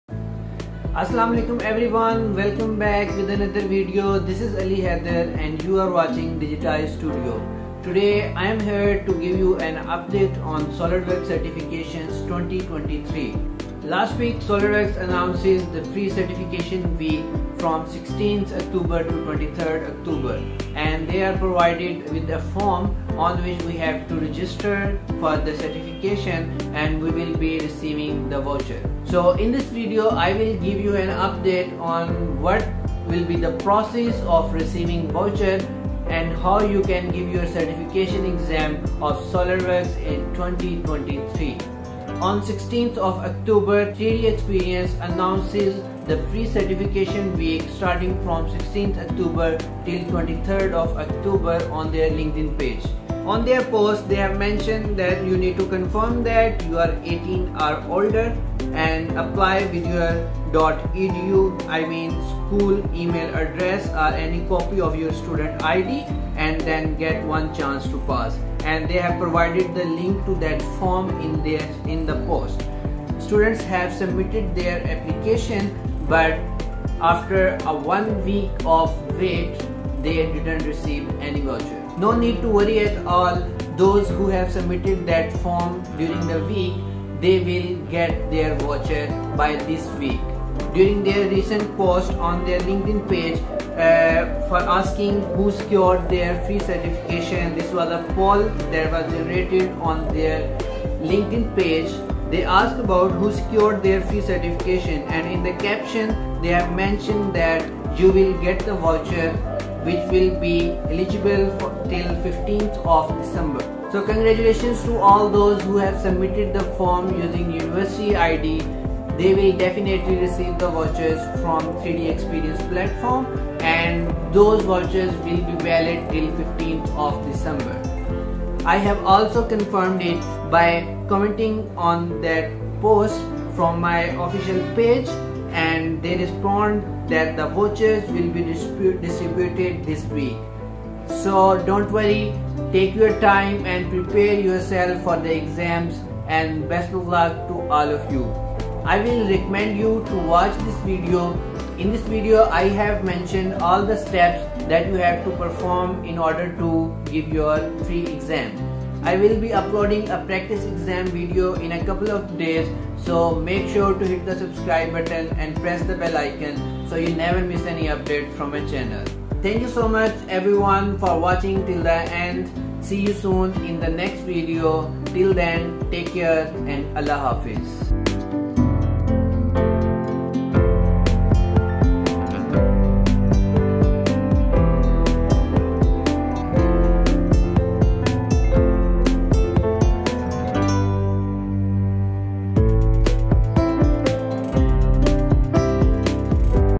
SolidWorks Certifications Free Week 2023 - Latest Update of Free Voucher - Free SolidWorks Exam
In this video, we bring you the latest updates on the Free Voucher giveaway, allowing you to take the SolidWorks Exam at absolutely no cost. Whether you're a student, a professional, or someone passionate about 3D design, this is an opportunity you don't want to miss.